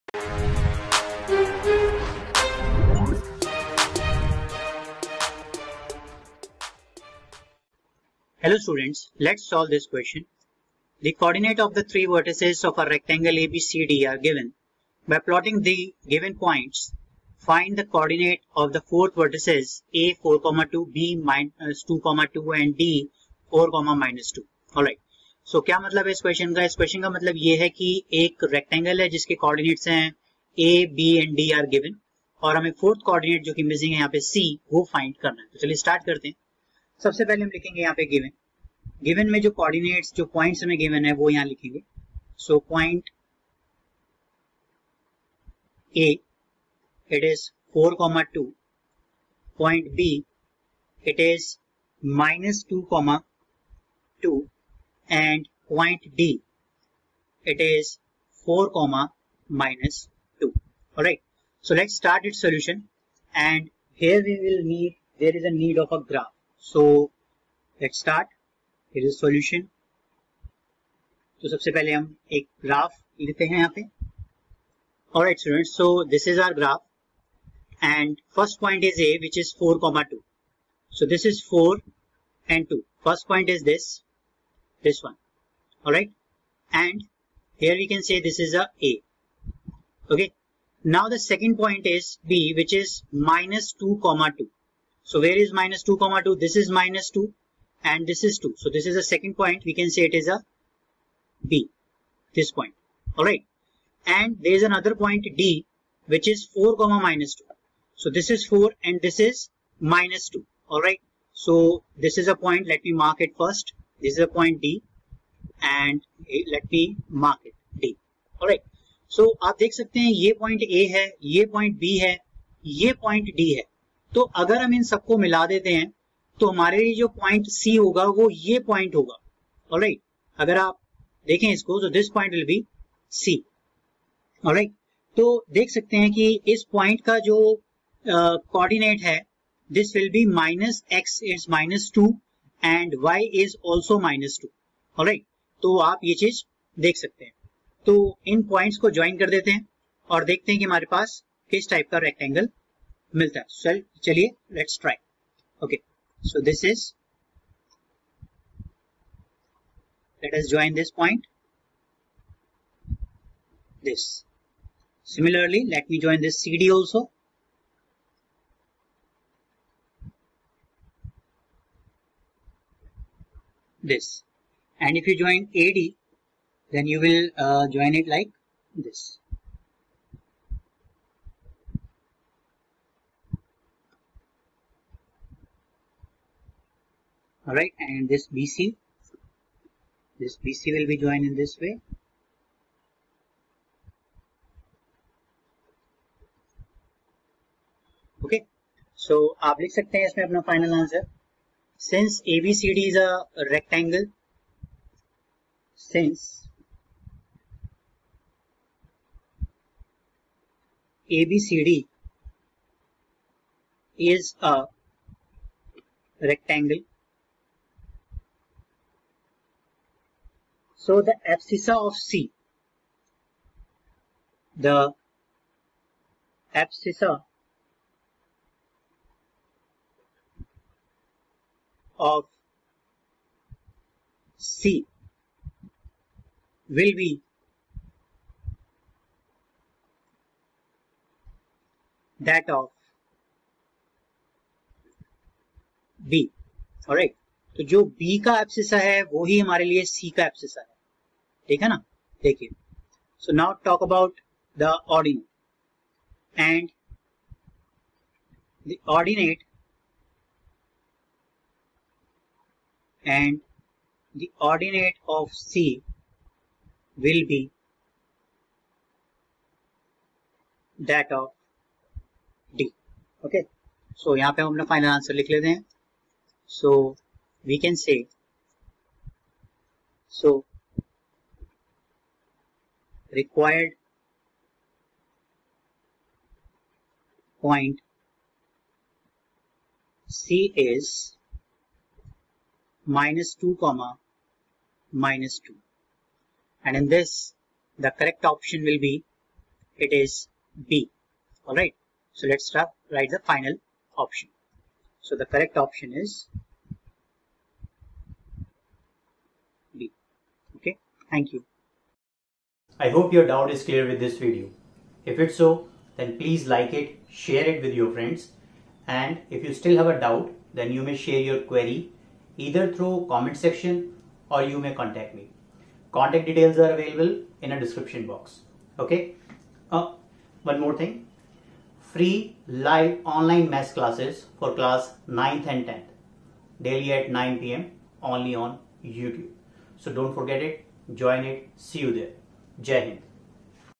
The coordinates of the three vertices of a rectangle ABCD are given. By Plotting the given points
Que.The coordinates of the three vertices of a rectangle ABCD are given. By Plotting the given points , find the co-ordinate of the fourth vertex: A(4,2), B(-2,2) and D(4,-2)
From the Chapter: Chapter -7 Coordinate Geometry Class 10th

1. Free Online Live Maths Classes Schedule
2. NCERT Textbook Video Solutions
3. Latest Updates Related To CBSE & Your Studies
4.Counselling Sessions
5.Free Study Material.